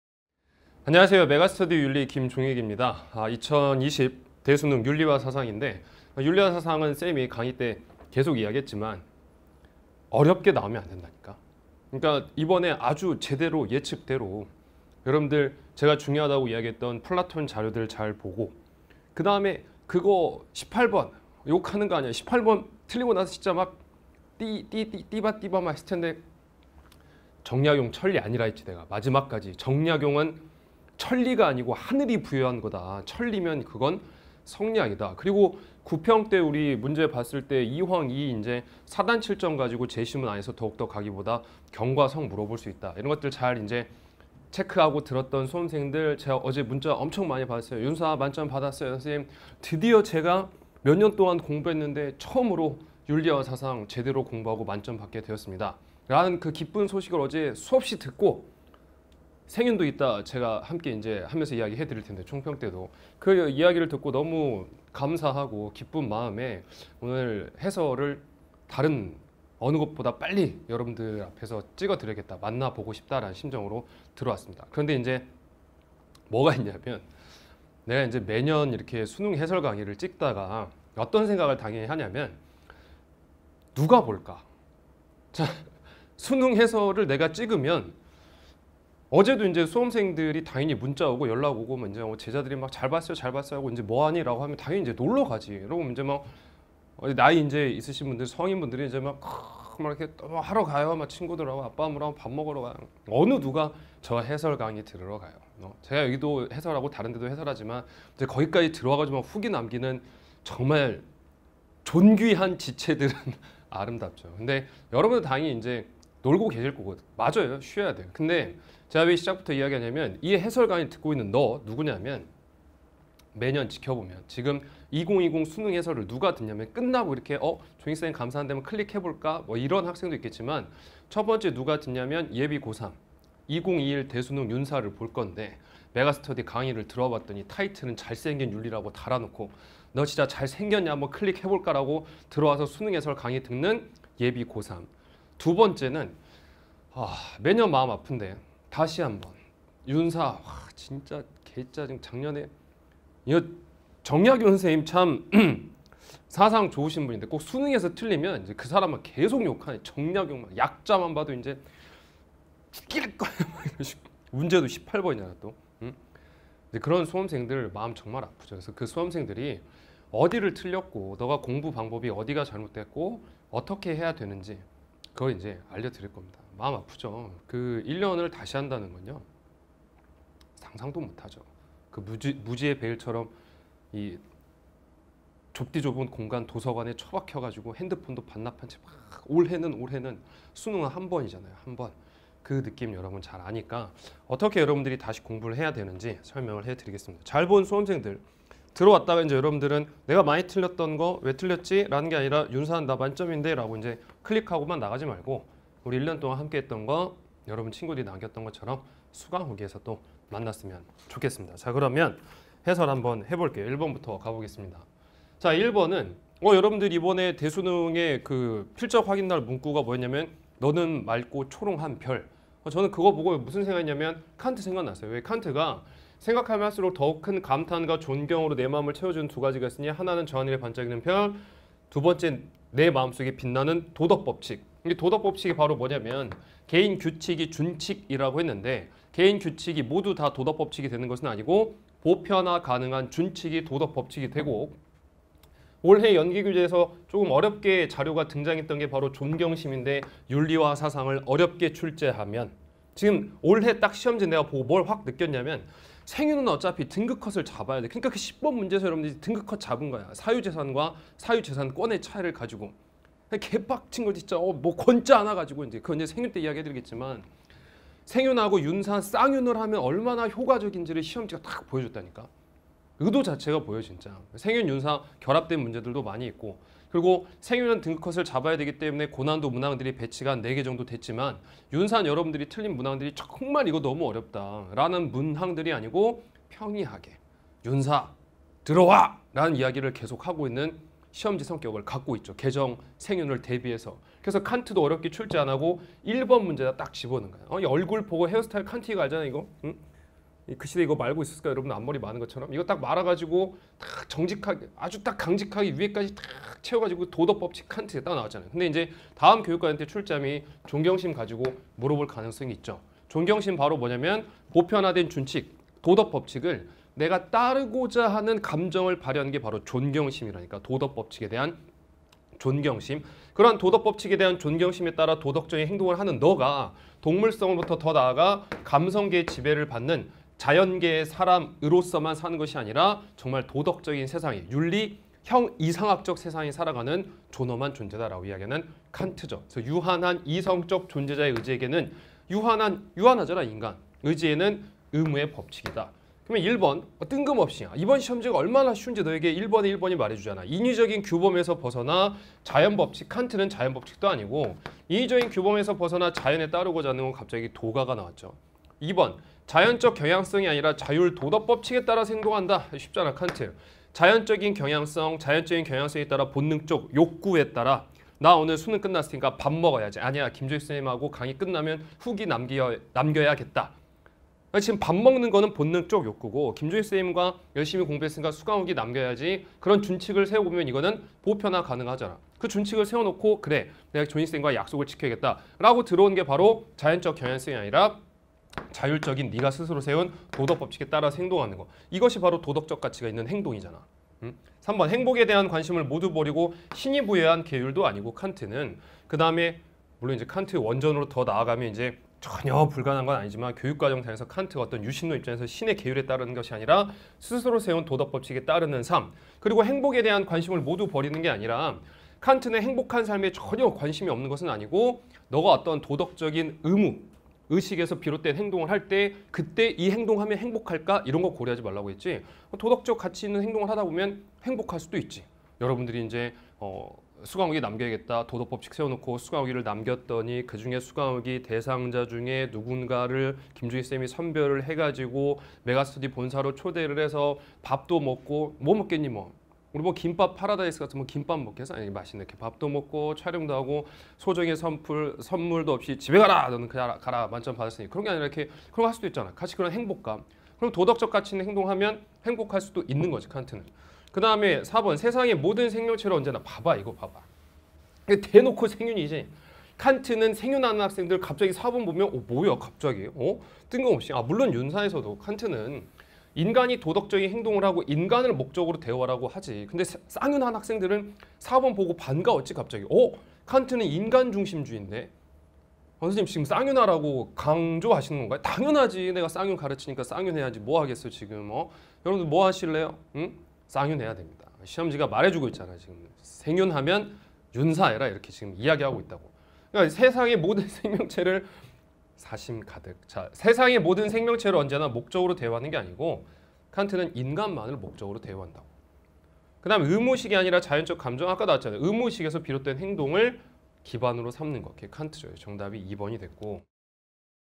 [메가스터디] 윤리 김종익 쌤 - 2020학년도 수능 윤리와 사상 해설강의
2019년 11월 14일 시행된 고3 수능 윤리와 사상
메가스터디 김종익 선생님의 해설강의입니다.